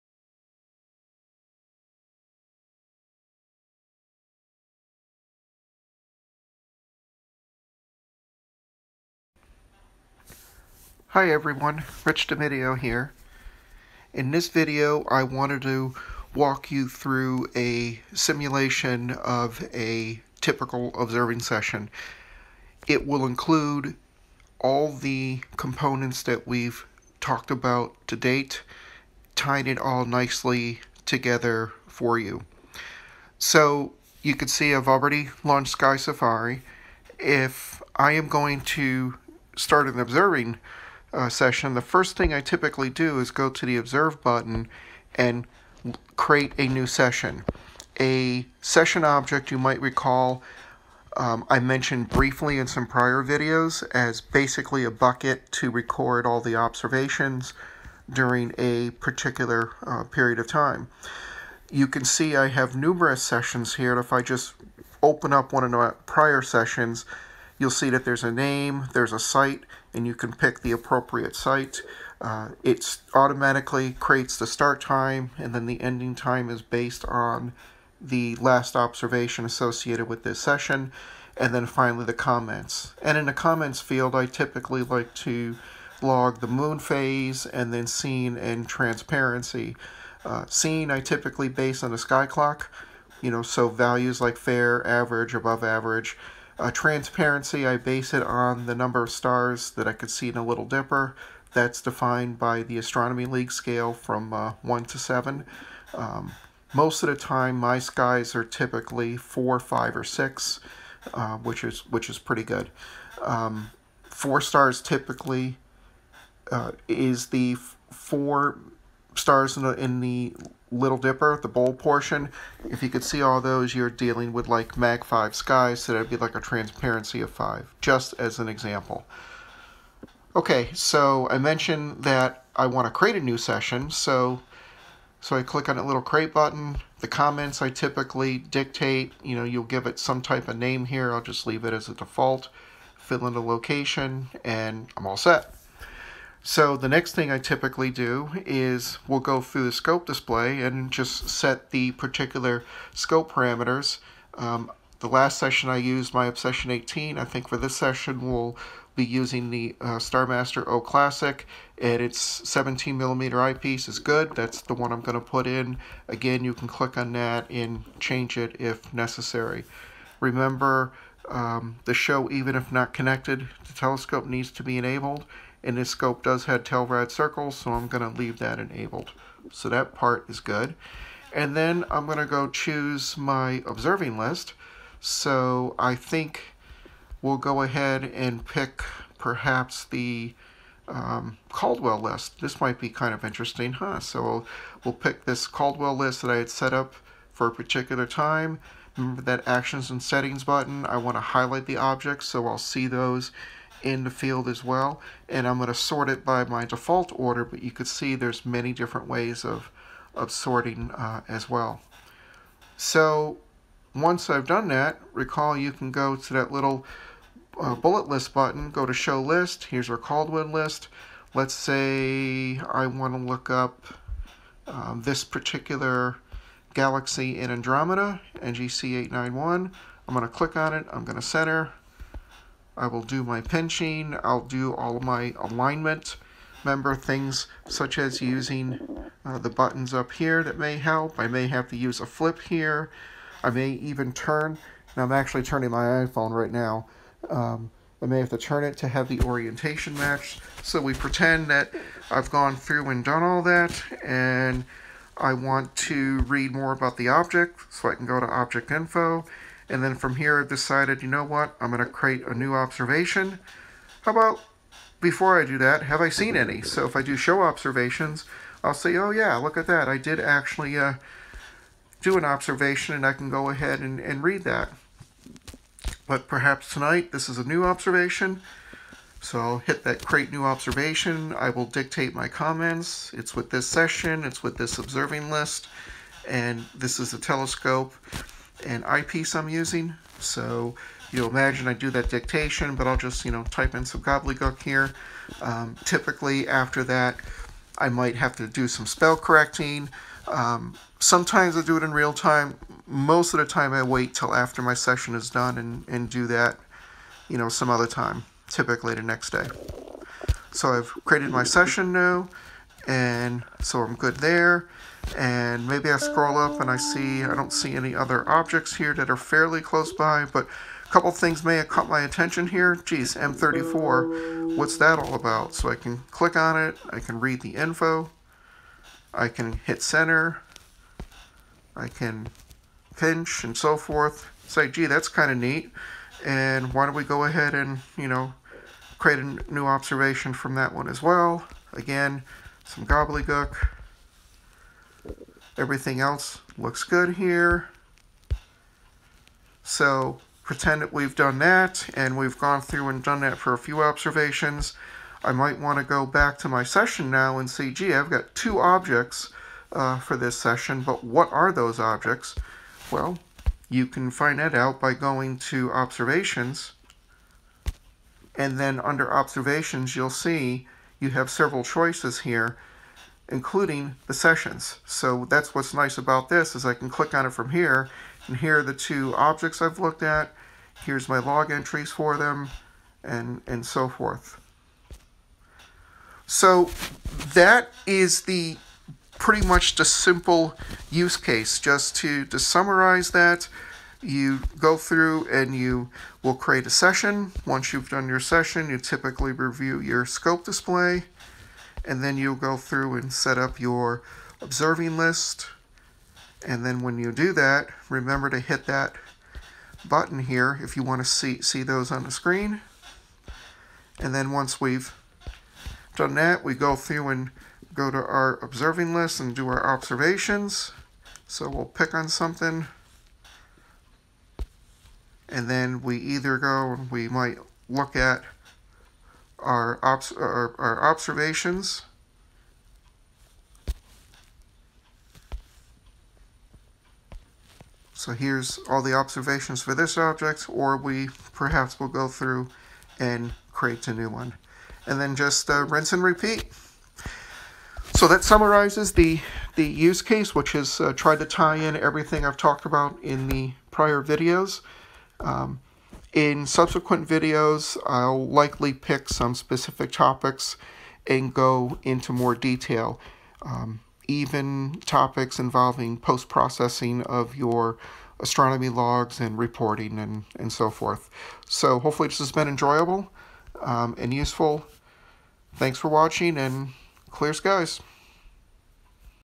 SkySafari Pro Part 5 Typical Session Simulation
This can be used in multiple ways.  If are new to the tool, then this one serves as both an introduction to the fundamental capabilities.Then, you can view the other lessons diving into the specific details.   If you are already familiar, then it serves as nice summary and refresher.